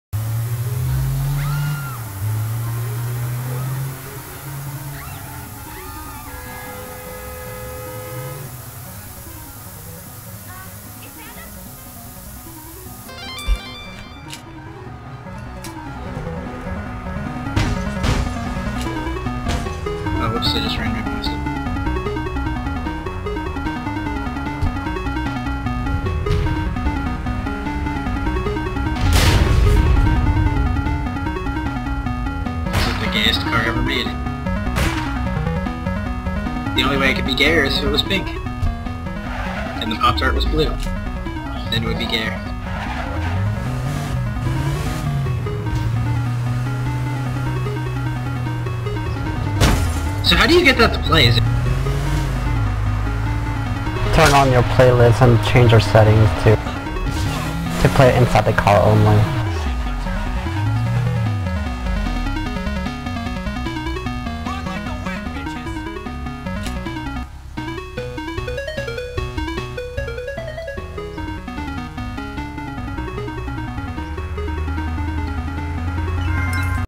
Cruisin' in the Nyan Mobile.
A friend of mine was able to scrap together this adorable hunk of metal on wheels. It even plays music.